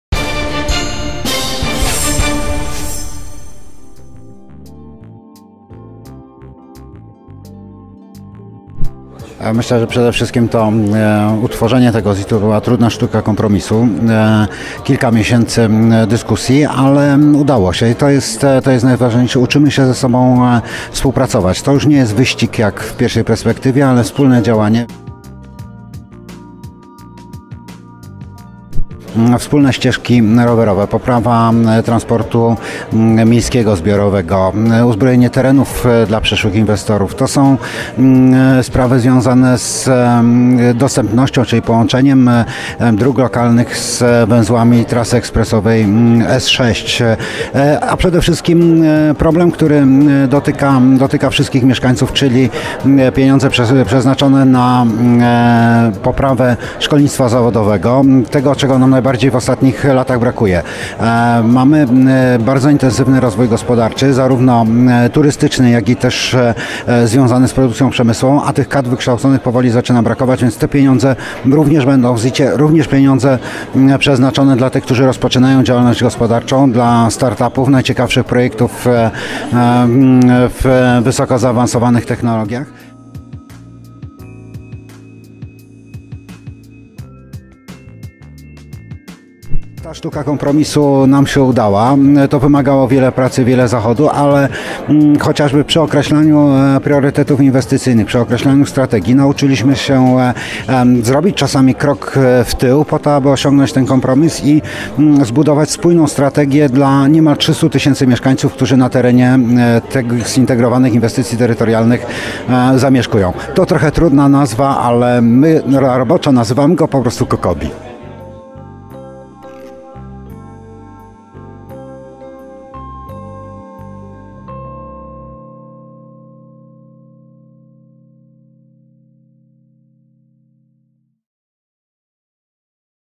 Piotr Jedliński mówi o ZIT dla KKBOF
Koszalińsko-Kołobrzesko-Białogardzki Obszar Funkcjonalny będzie korzystał z mechanizmu Zintegrowanych Inwestycji Terytorialnych. O tym, jak samorządy się dogadały i na jakie inwestycje przeznaczą fundusze unijne, mówi prezydent Koszalina - Piotr Jedliński.